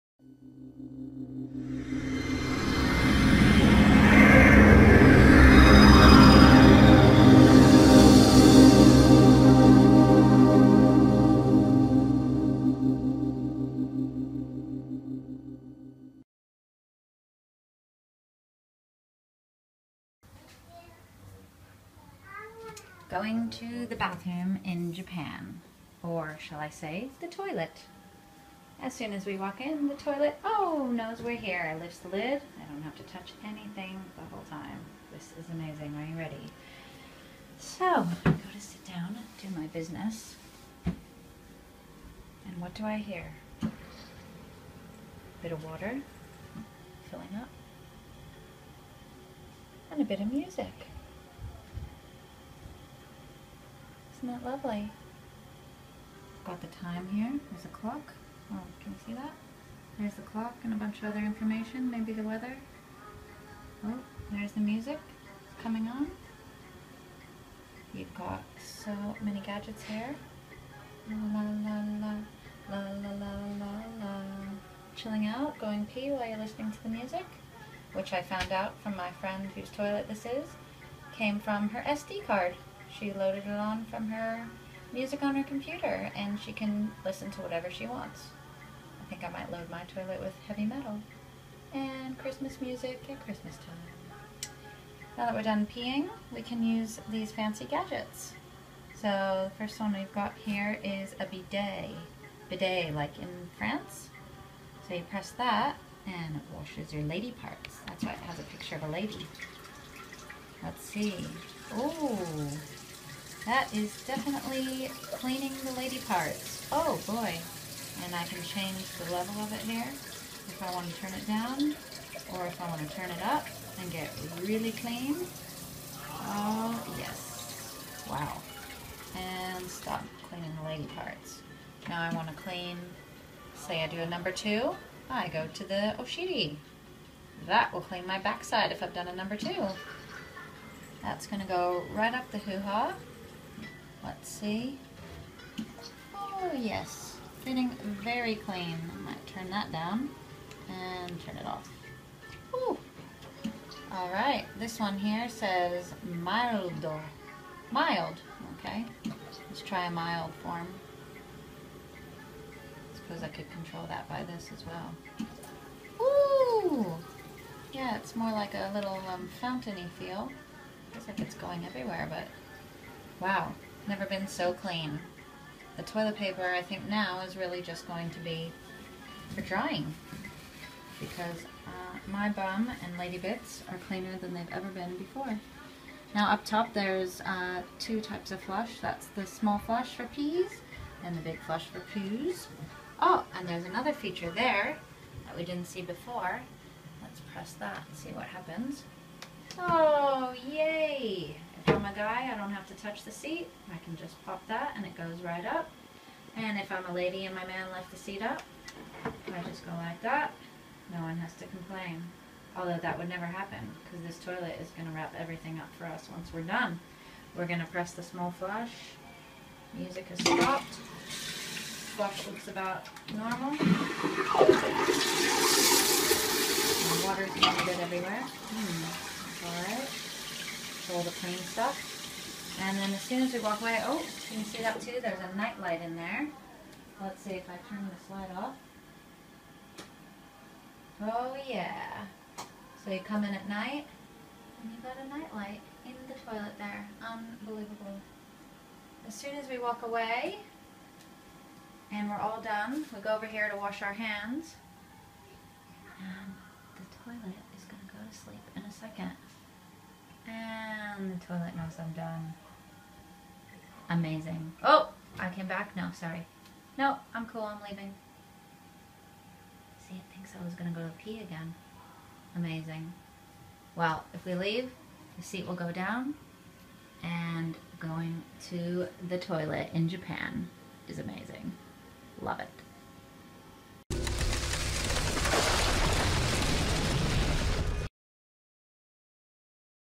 The Japanese toilet that warms, cleans and entertains you!
Join Bex on a Japanese toilet! You may have heard about or seen some of the amazing Japanese inventions, and their toilets are no exception. It's like sitting on a space ship! Traditional Japanese toilets are squat toilets, aka hole in the floor, which are amazing for the fully emptying the bowels. These days, most Japanese now use western toilets, and though the benefits of the squat position are lost, they've made up for it with toilets that provide exceptional service. Toilets in Japan can warm your bum, clean you thoroughly, play music for you, release pleasant scents, and raise the lid and seat so you never have to touch it.